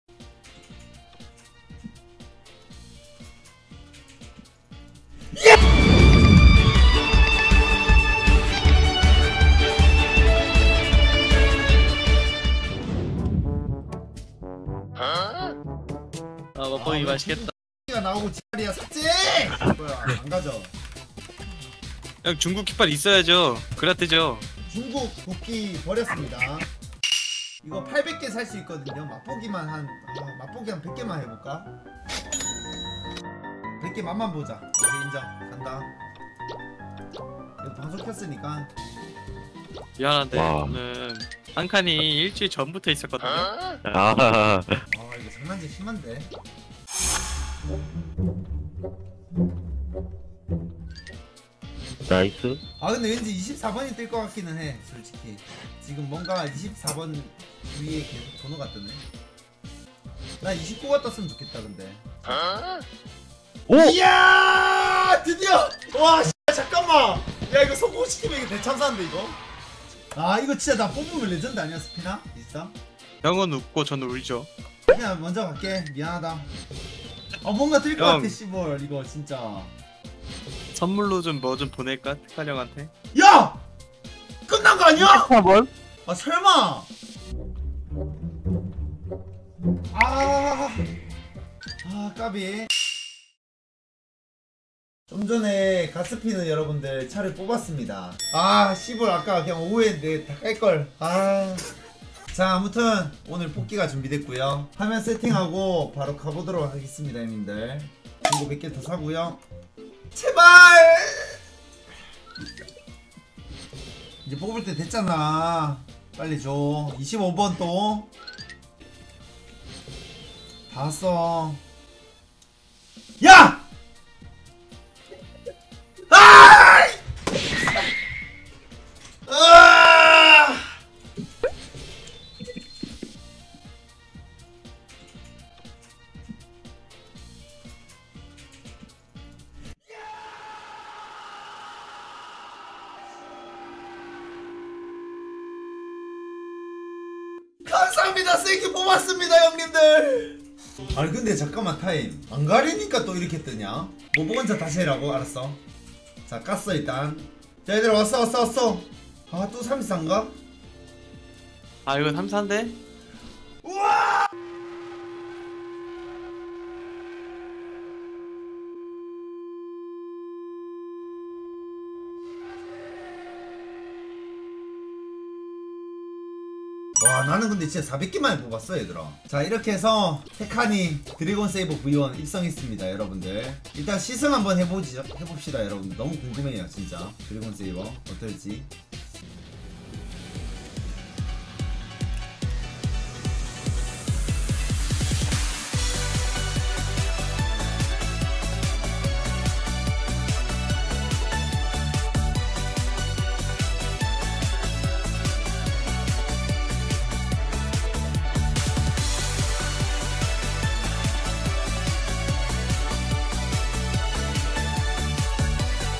"27만원 현질" 드래곤 세이버 V1 드디어 뽑았습니다!
드래곤 세이버 V1 뽑기 성공!